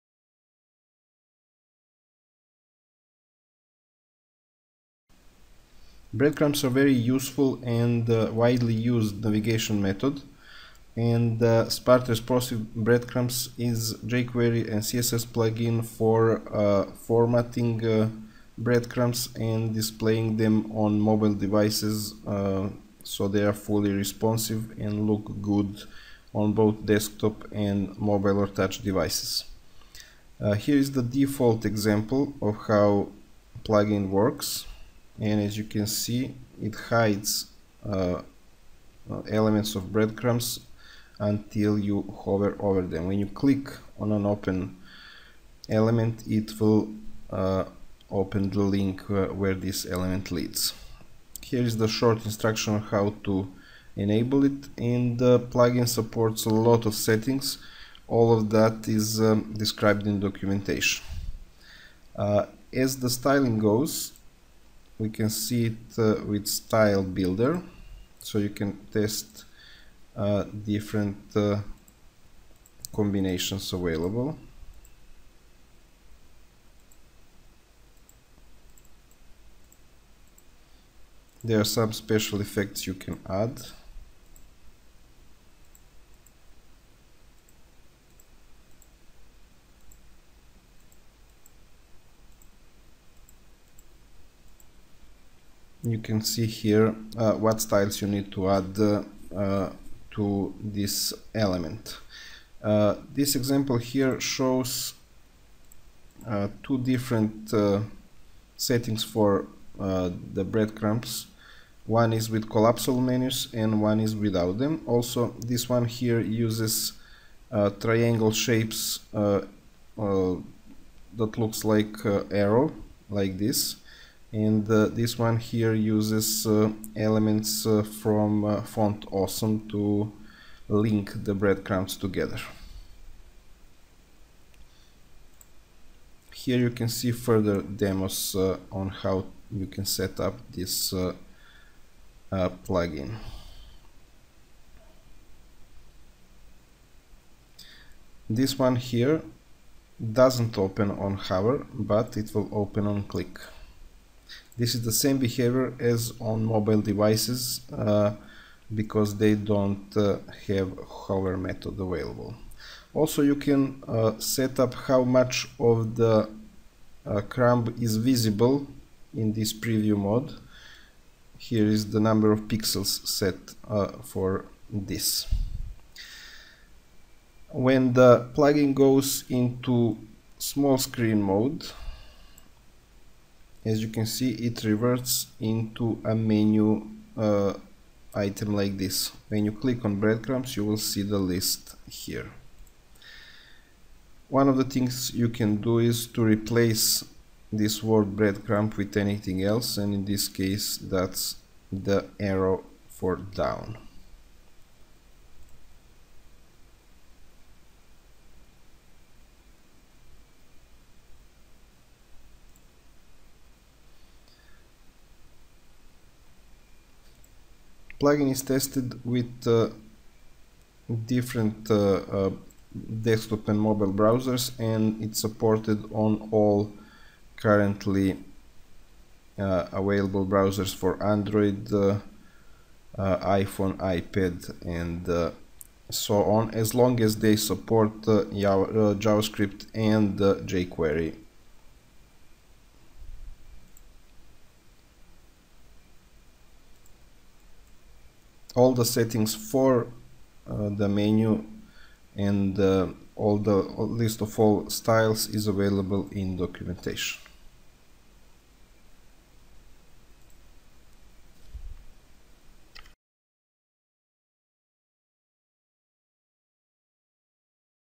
Smart Responsive Breadcrumbs
Smart Responsive Breadcrumbs is powerful breadcrumbs navigation solution that will work with mobile devices and different screen sizes. Breadcrumbs rely on CSS media queries to modify and display for different resolutions. For less than 480px, menu changes from horizontal navigation into vertical and gets hidden behind the menu item.